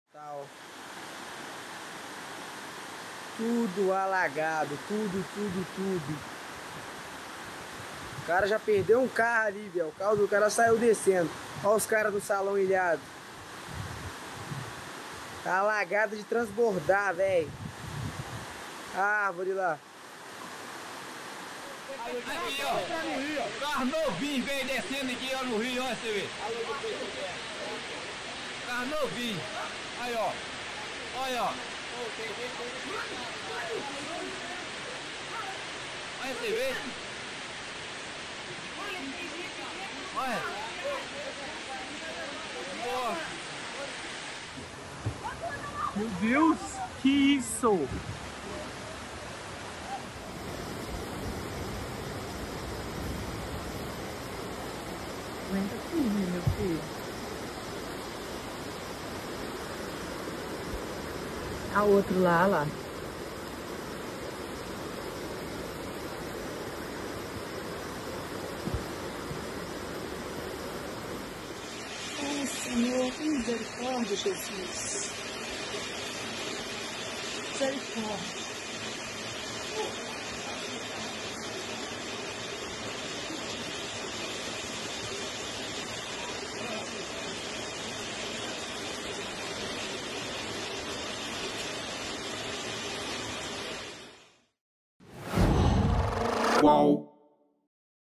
Chuvas arrastam carros e causam inundações em Belo Horizonte
Fortes chuvas que atingem Belo Horizonte desde as 17h desta quarta-feira (7) causaram inundações, arrastaram carros e derrubaram até mesmo o teto de uma delegacia na capital mineira.